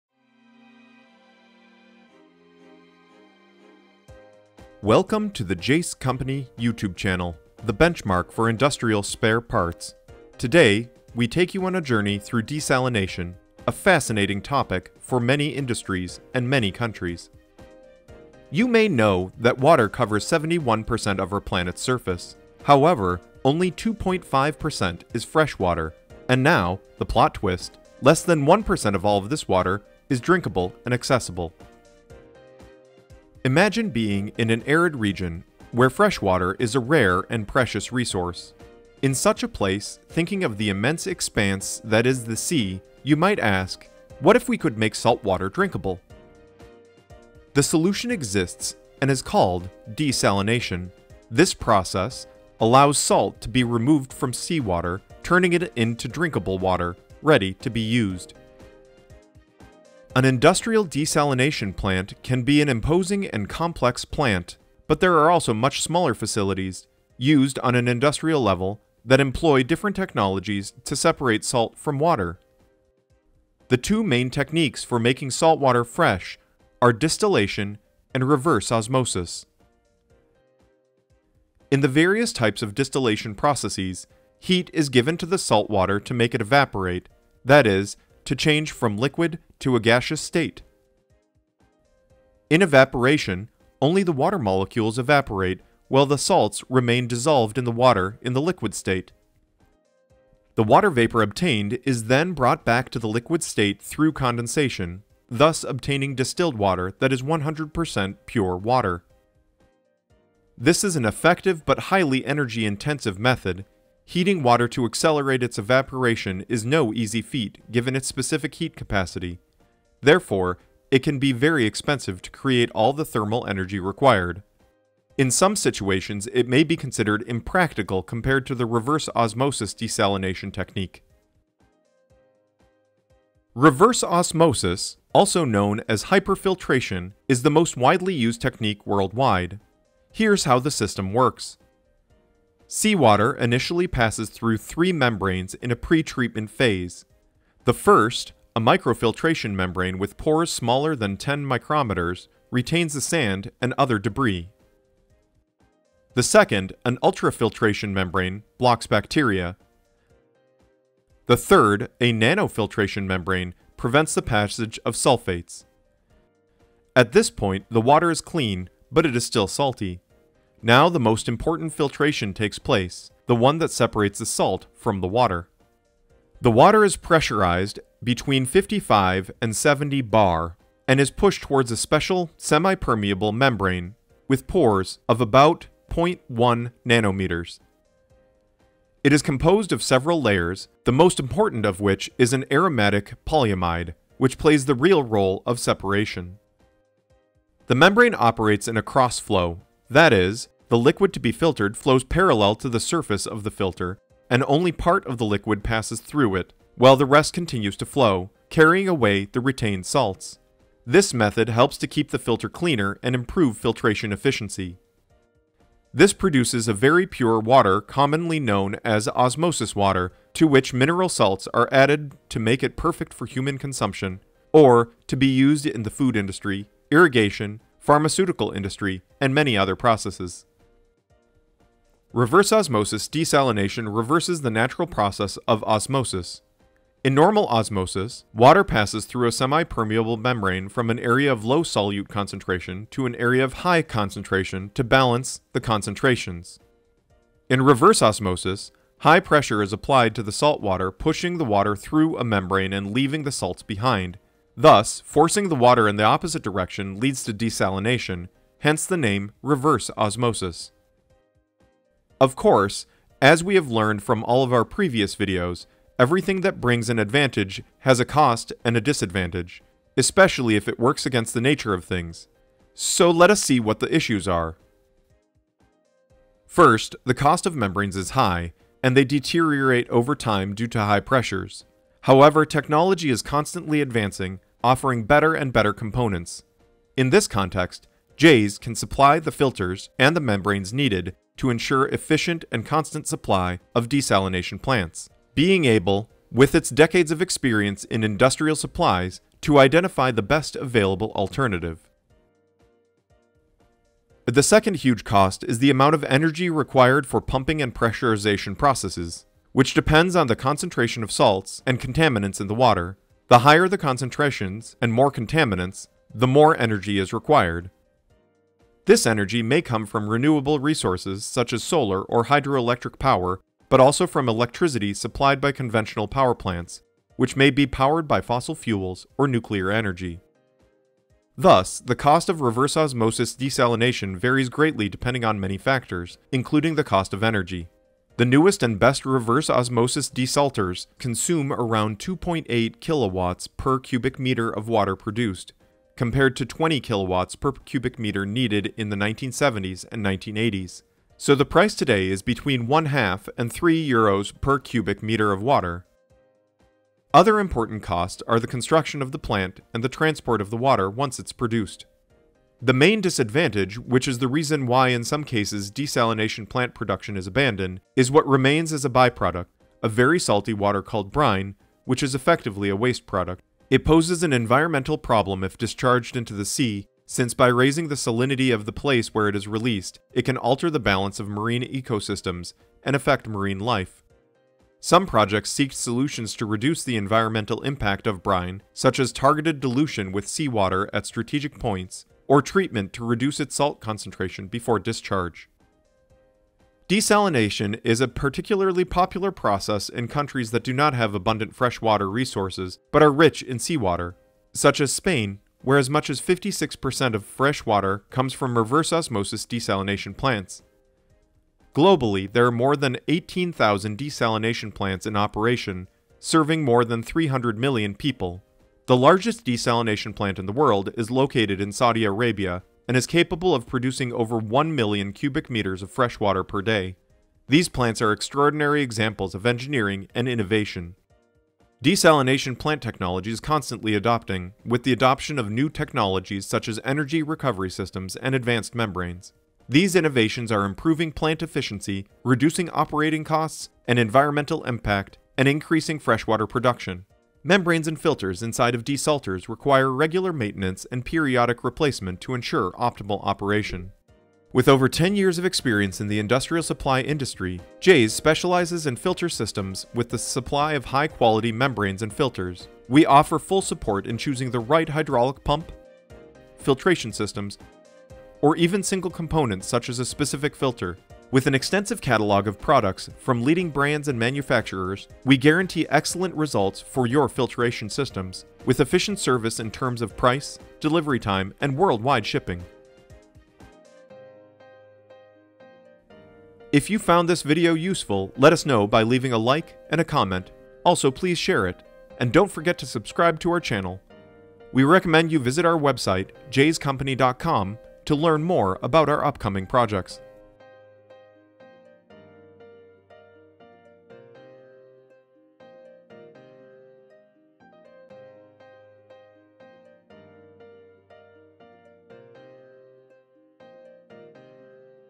INDUSTRIAL DESALINATION PLANTS: How They Work and What is Desalination?
Welcome to Jaes company's YouTube channel, the benchmark for industrial spare parts. Today, we take you on a journey through desalination, a fascinating topic for many industries and many countries!

You may know that water covers 71% of our planet's surface. However, only 2.5% is fresh water and now the plot twist: less than 1% of all this water is drinkable and accessible.
Imagine being in an arid region where fresh water is a rare and precious resource. In such a place, thinking of the immense expanse that is the sea, you might ask: "What if we could make salt water drinkable?"
The solution exists and it is called desalination. This process allows salt to be removed from seawater, turning it into drinkable water, ready to be used.

An industrial desalination plants can be an imposing and complex plant, but there are also much smaller facilities, used on an industrial level, that employ different technologies to separate salt from water.
The two main techniques for making salt water fresh are distillation and reverse osmosis.

In the various types of distillation processes, heat is given to the salt water to make it evaporate, that is, to change it from the liquid to the gaseous state. In evaporation, only the water molecules evaporate, while the salts remain dissolved in the water in the liquid state.
The water vapour obtained is then, through condensation, brought back to the liquid state, thus obtaining distilled water, that is 100% pure water.
This is an effective, but highly energy-intensive method (heating water to accelerate its evaporation is no easy feat, given its specific heat capacity). Therefore, it can be very expensive to create all the thermal energy required.
Therefore, in some situations it may be considered impractical compared to the reverse osmosis desalination technique.

The reverse osmosis technique, also known as hyperfiltration, is the most widely used technique worldwide.
Here is how the system works:
Sea water initially passes through three membranes in a pre-treatment phase.
The first, a microfiltration membrane with pores smaller than 10 micrometres, retains the sand.
The second, an ultrafiltration membrane, blocks bacteria.
The third, a nanofiltration membrane, prevents the passage of sulphates.
At this point, the water is clean, but it is still salty! Now the most important filtration takes place, the one that separates the salt from the water.
The water is pressurised between 55 and 70 bar, and pushed towards a special semipermeable membrane with pores of about 0.1 nanometres.
It is composed of several layers, the most important of which is aromatic polyamide, which plays the real role of separation.
This membrane operates in cross-flow, that is,  the liquid to be filtered flows parallel to the surface of the filter and only part of the liquid passes through it, while the rest continues to flow, carrying away the retained salts. This method helps to keep the filter cleaner and improve filtration efficiency.
This produces a very pure water commonly called osmosis water to which mineral salts are added to make it perfect for human consumption, or to be used in the food industry, irrigation, pharmaceutical industry and many other processes.

Reverse osmosis desalination reverses the natural process of osmosis. In normal osmosis, water passes through a semi-permeable membrane from an area of low solute concentration to an area of high concentration to balance the concentrations. In reverse osmosis, high pressure is applied to the salt water, pushing the water through the membrane and leaving the salts behind. This forcing pure water in the opposite direction leads to desalination, hence the name 'reverse osmosis'.

Of course, as we have learned from all our previous videos, everything that brings an advantage has a cost and a disadvantage. Especially if it works against the nature of things. Let us see what the issues are.